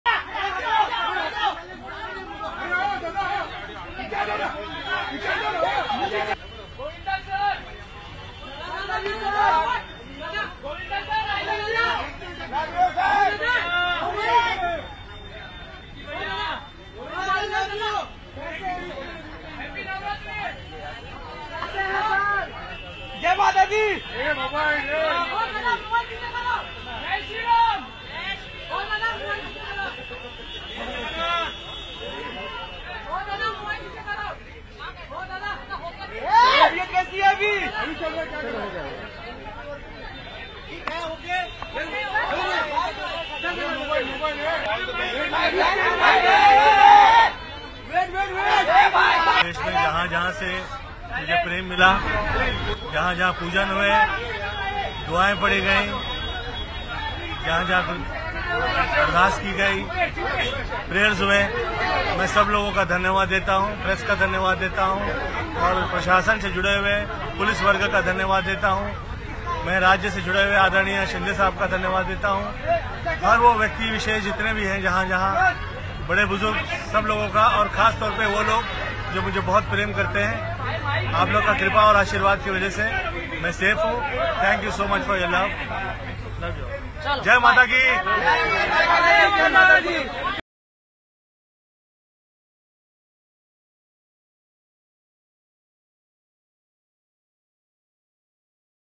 पैर पर प्लास्टर...व्हील चेयर पर Hospital से बाहर निकले Govinda..., फैंस का किया शुक्रिया
बीते मंगलवार को गोविंदा के पैर में अचानक उनकी ही रिवॉल्वर से गोली लग गई थी...जिसके बाद उन्हें क्रिटीकेयर हॉस्पिटल में एडमिट किया गया था...आज गोविंदा को अस्पताल से छुट्टी मिल गई है...आप देख सकते हैं कि उनके पैर पर प्लास्टर है और वो व्हील चेयर पर अस्पताल से बाहर आए..जिसके बाद उन्हें तमाम मिडियाकर्मियों ने घेर लिया…यहां गोविंदा ने कहा कि मैं आप सभी की दुआओं से ठीक हूं...